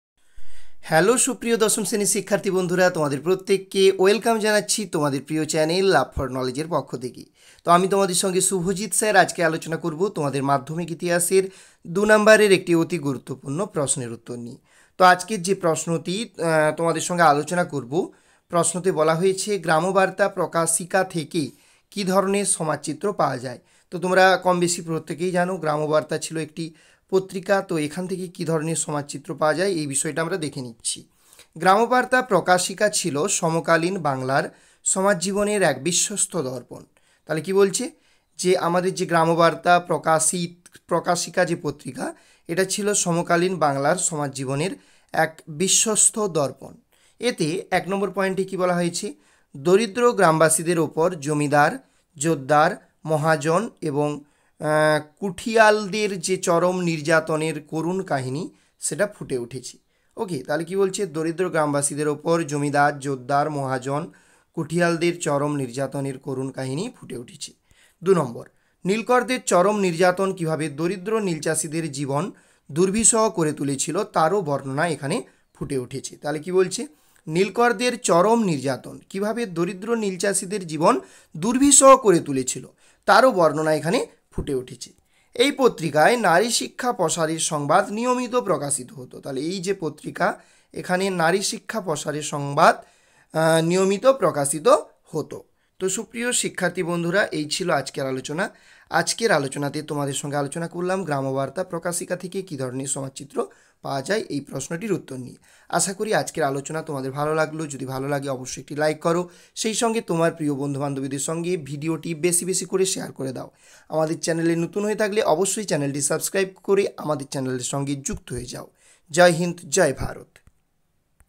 মাধ্যমিক ইতিহাস। গ্রামবার্তা প্রকাশিকা থেকে কি ধরনের সমাজ চিত্র পাওয়া যায়? ২ নাম্বারের প্রশ্নোত্তর
মাধ্যমিক ইতিহাস। গ্রামবার্তা প্রকাশিকা থেকে কি ধরনের সমাজ চিত্র পাওয়া যায়? ২ নাম্বারের প্রশ্নোত্তর। loveforknowledge. Discussed By Suvajit Sir.